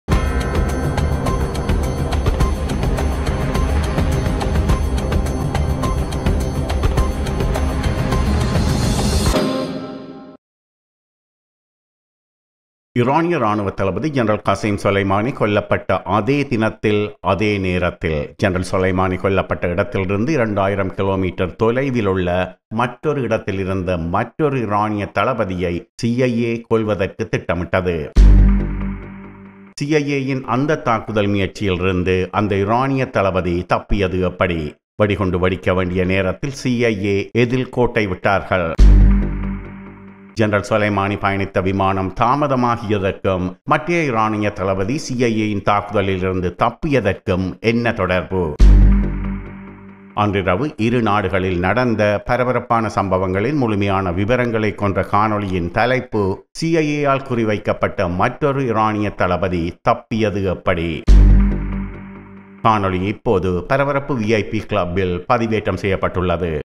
Promo: சி.ஐ.ஏ.யால் குறிவைக்கப்பட்ட மற்றொரு ஈரானியத் தளபதி தப்பியது எப்படி? | Paraparapu VIP Club
பரபரப்பு VIP Club-ல் இணைந்து கொள்ள விங்க்